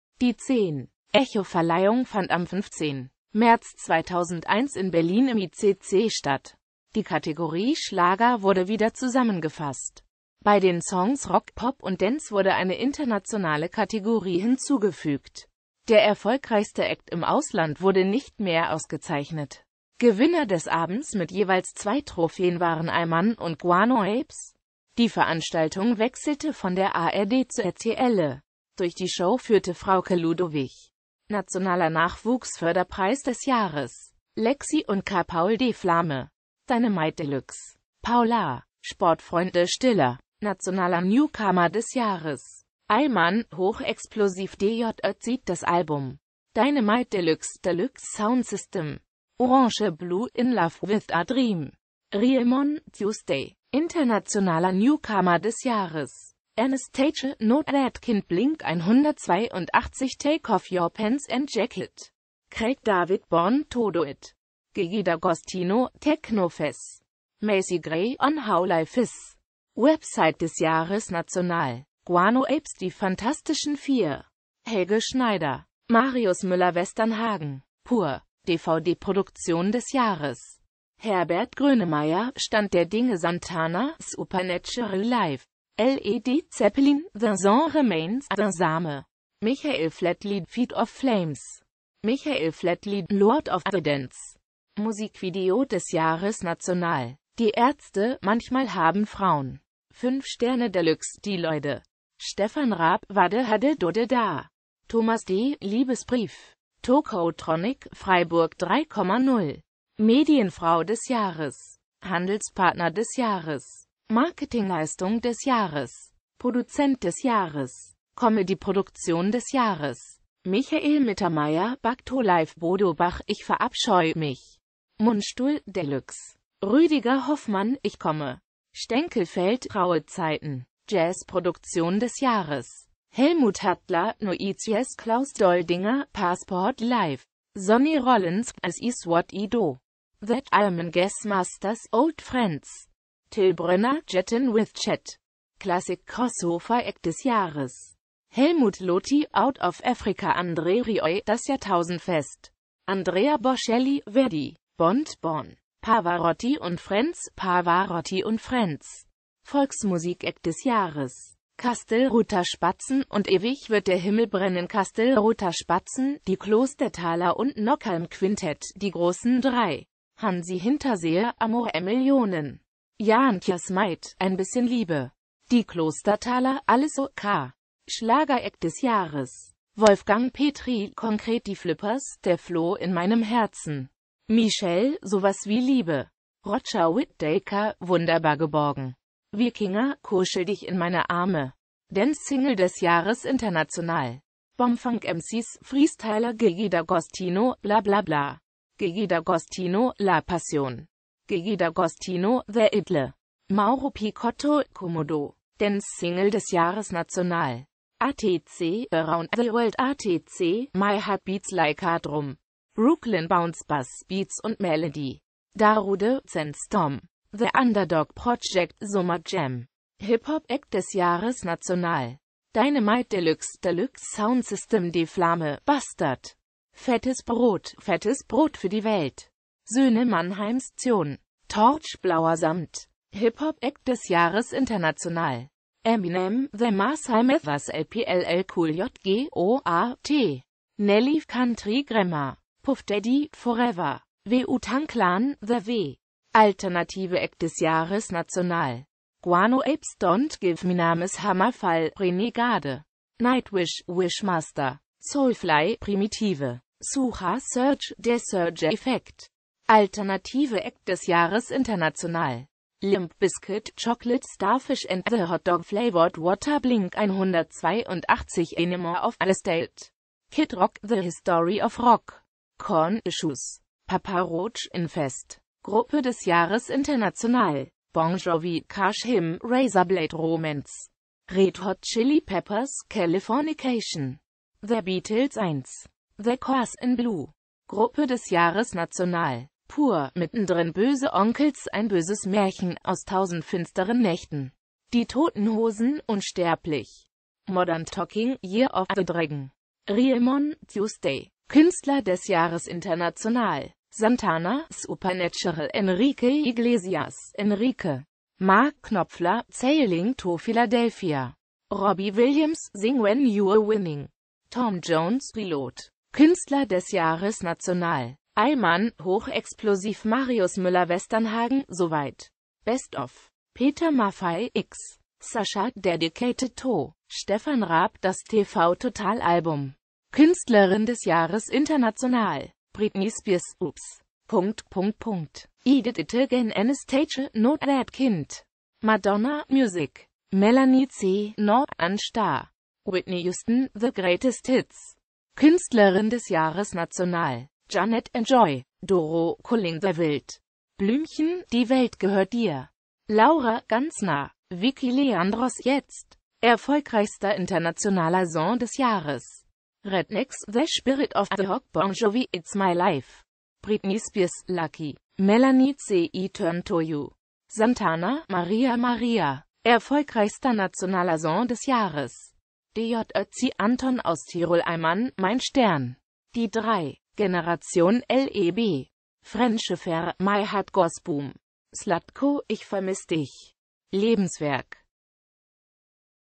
Echoverleihung 2001
Vielen Dank für Ihre Unterstützung:

Die 10.Echoverleihung fand am 15.März 2001 in Berlin im ICC statt.Die Kategorie Schlager wurde wieder zusammengefasst.